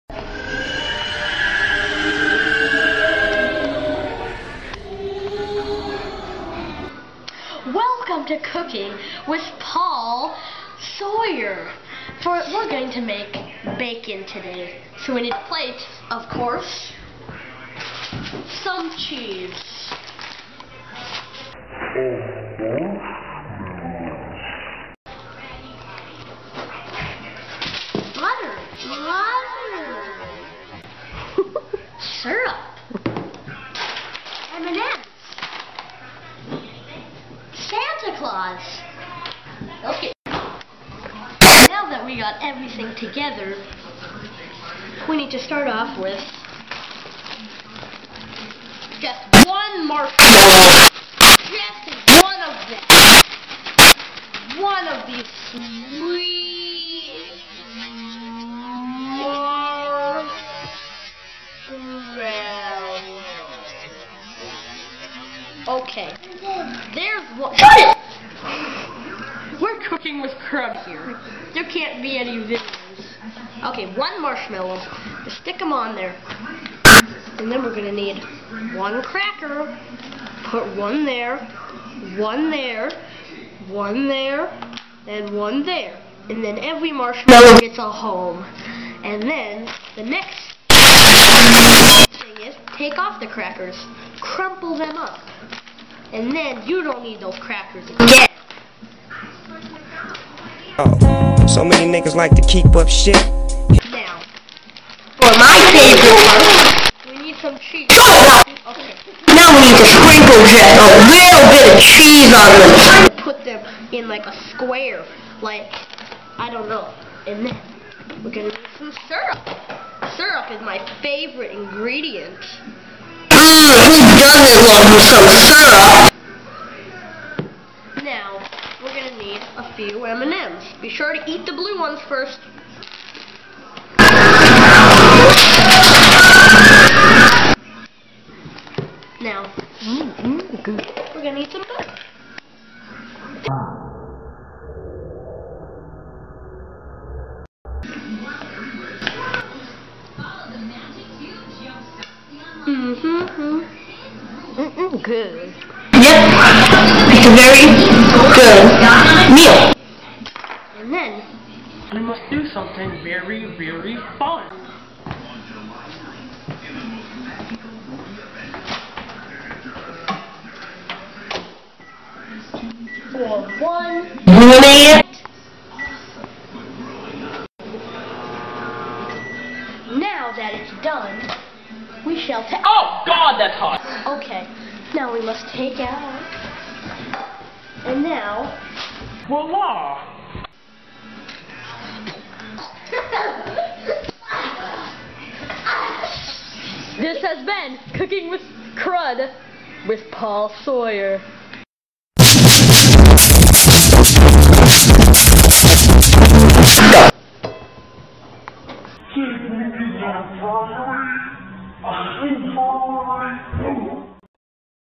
[YTP] pro cook paul saywer (food network show)
this show was officially pitched to Food Network in 1866 by pawl serper... needless to say it was the best show in the history of food network

any music or shiz is not owned by me i mean do you think im the snoopster